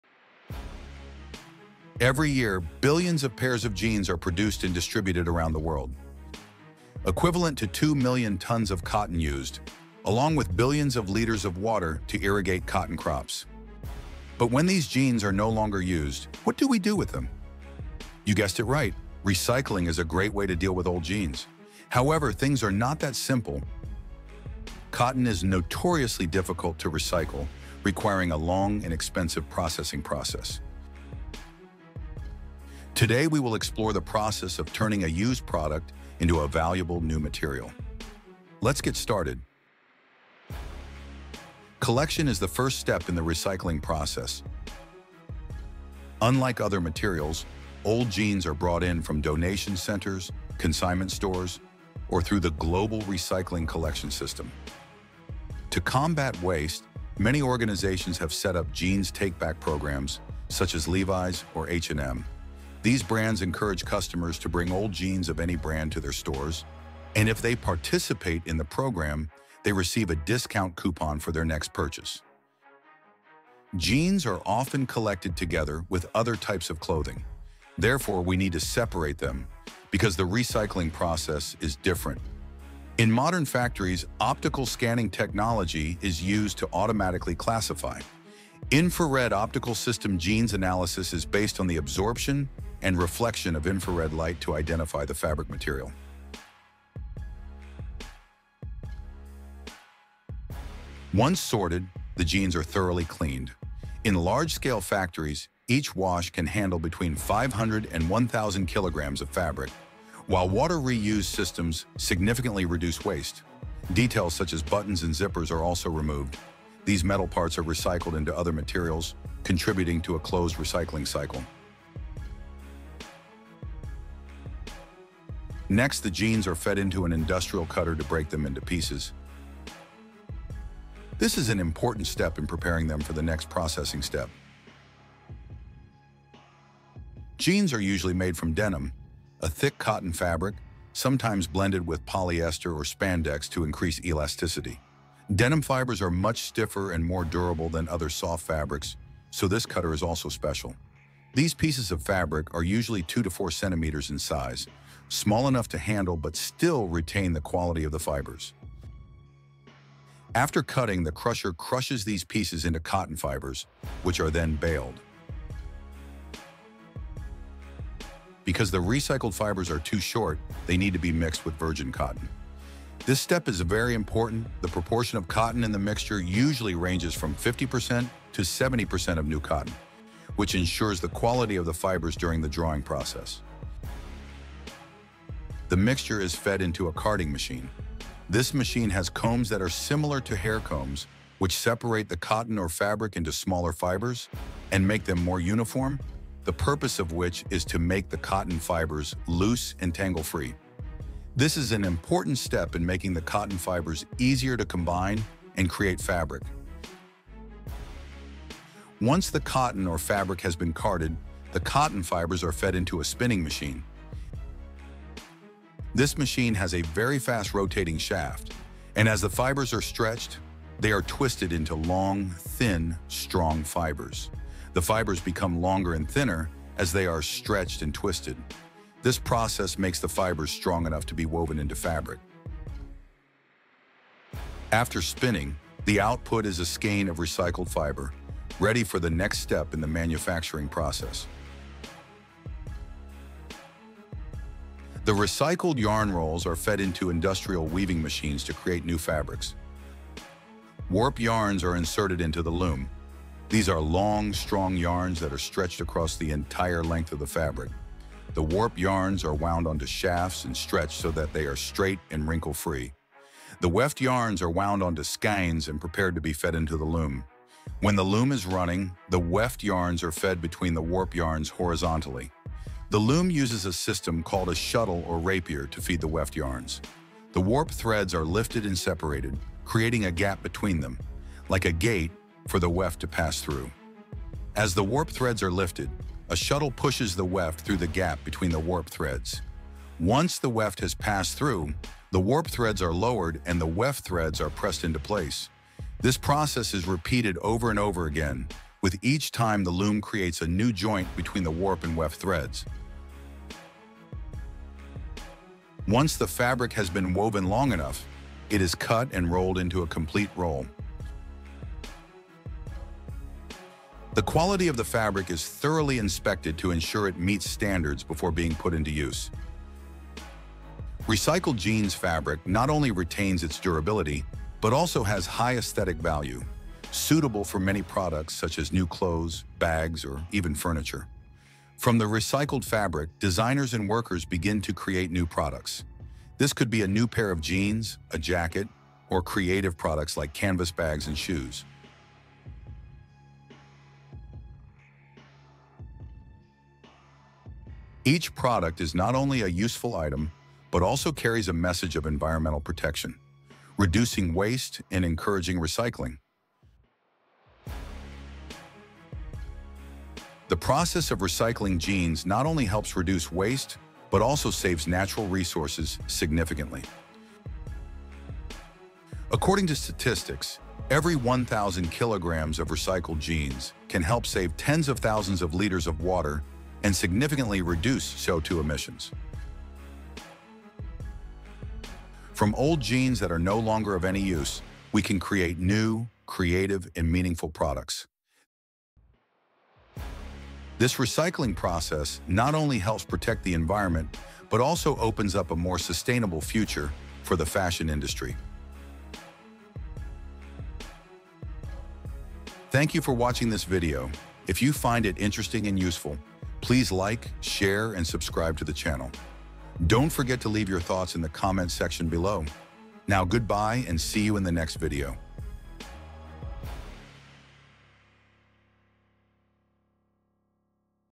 From Waste to Resources: Amazing process Recycling Milions Old Jeans
Every year, billions of pairs of jeans are produced and distributed around the world, equivalent to 2 million tons of cotton used, along with billions of liters of water to irrigate cotton crops. But when these jeans are no longer used, what do we do with them?
Today, we will explore the recycling process turning a used product into a valuable new material. Let's get started.

We does not own the rights to these video clips.

✅ Thank you everyone for watching our video!